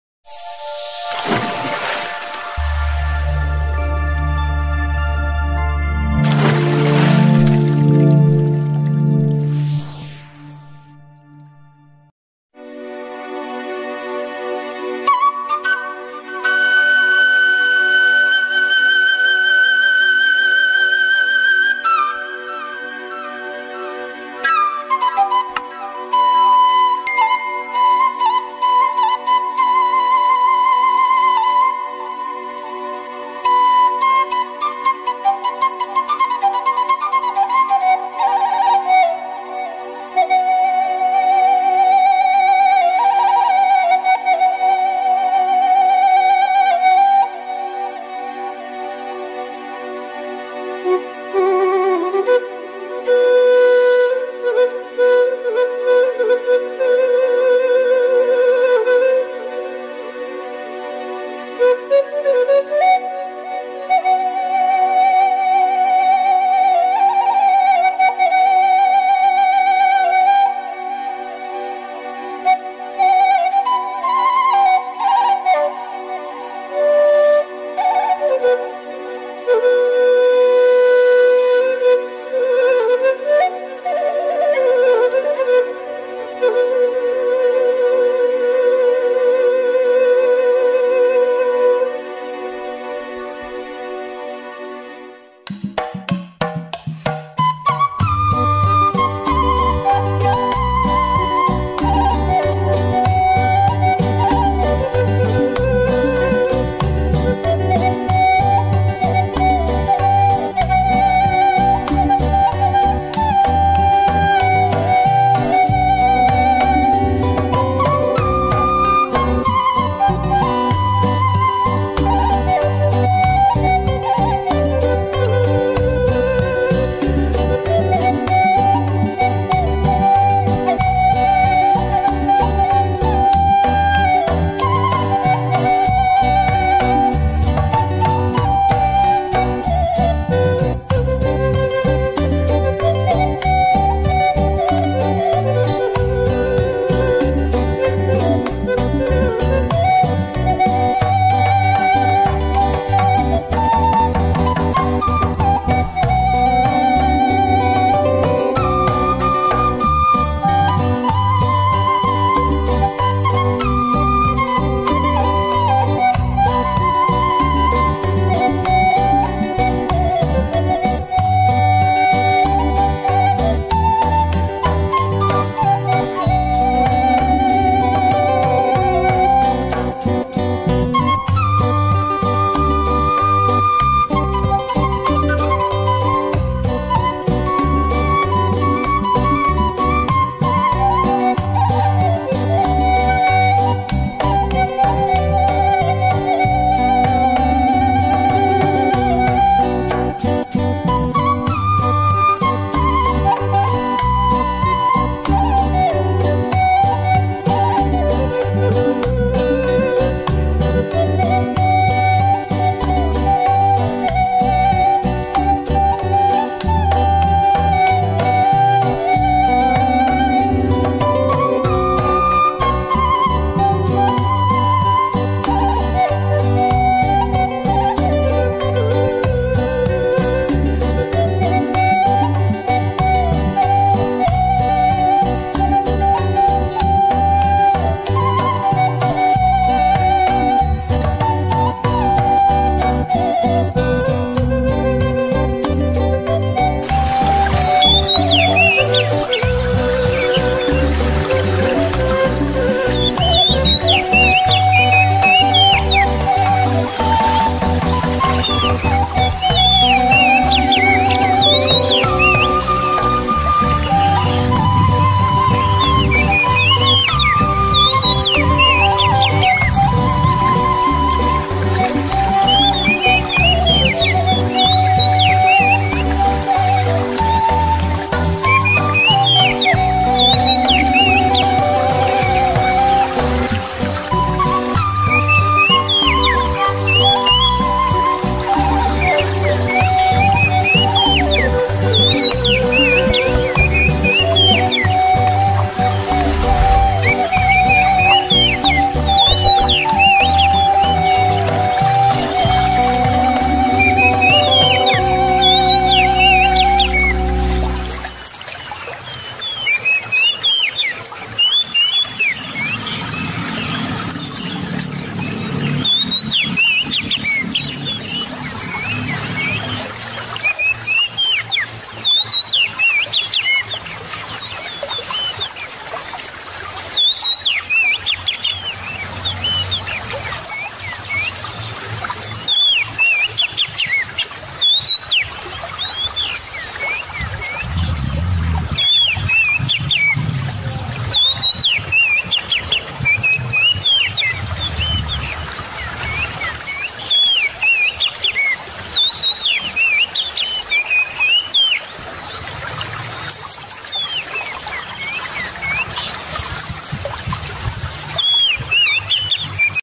Pagi di kawasan bukit lestari
menikmati keindahan alam dan udara sejuk di kawasan bukit lestari kelurahan lewiliang, kawalu kota tasikmalaya